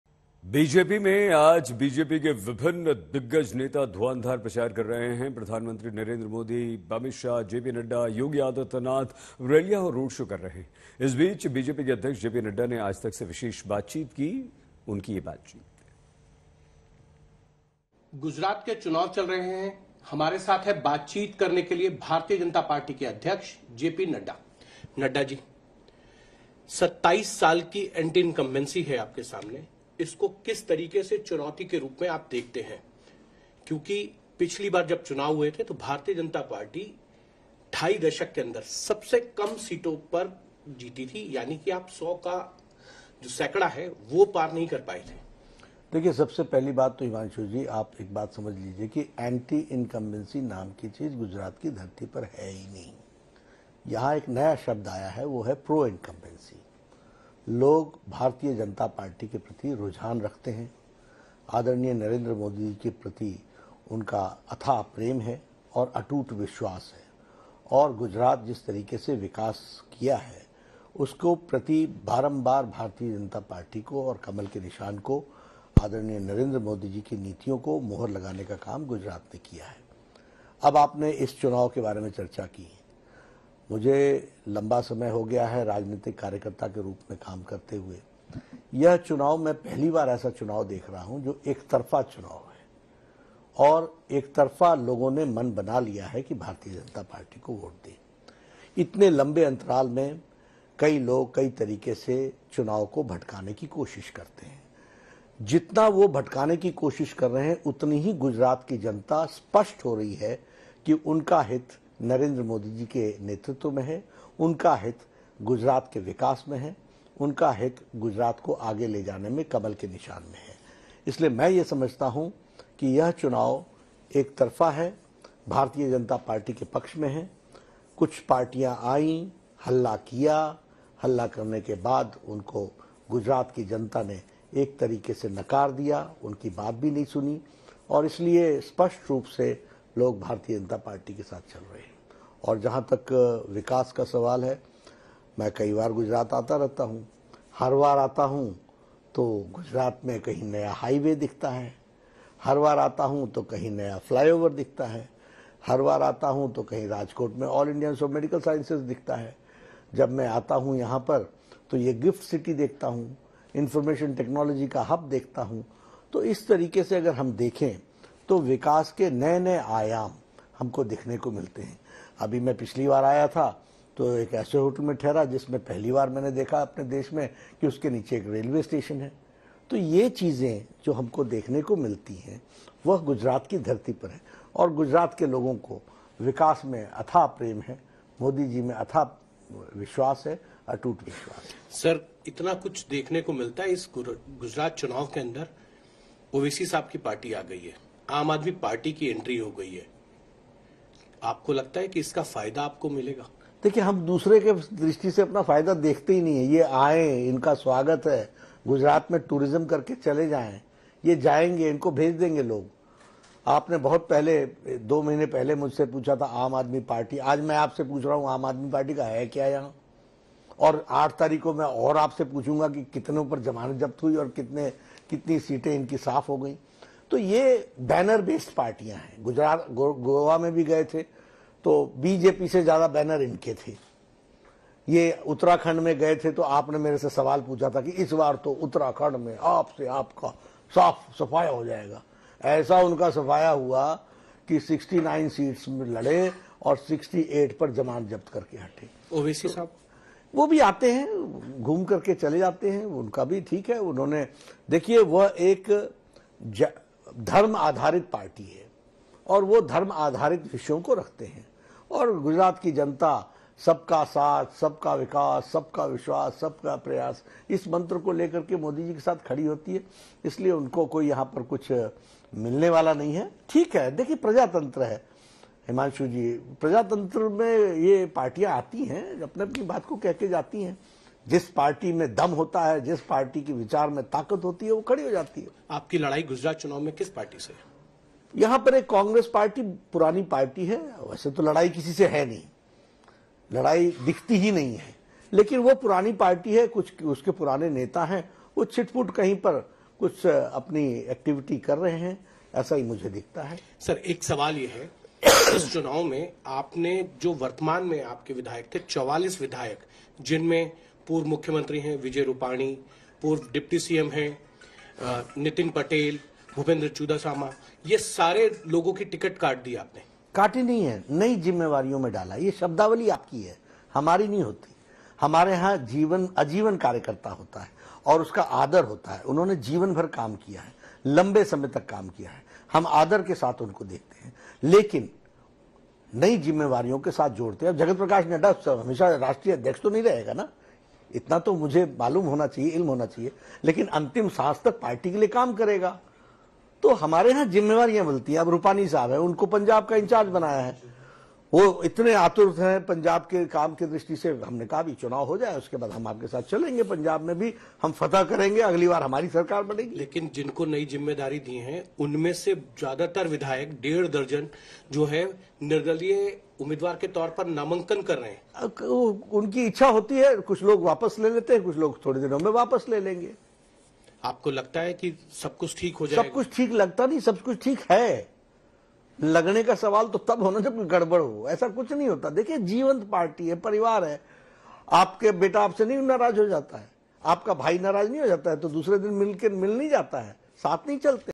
JP Nadda On Gujarat Elections 2022: जेपी नड्डा ने कहा- Gujarat में एकतरफा मुकाबला
Gujarat Elections को लेकर जेपी नड्डा ने कहा कि गुजरात में बीजेपी की जीत पक्की है. गुजरात की जनता को बीजेपी पर अटूट विश्वास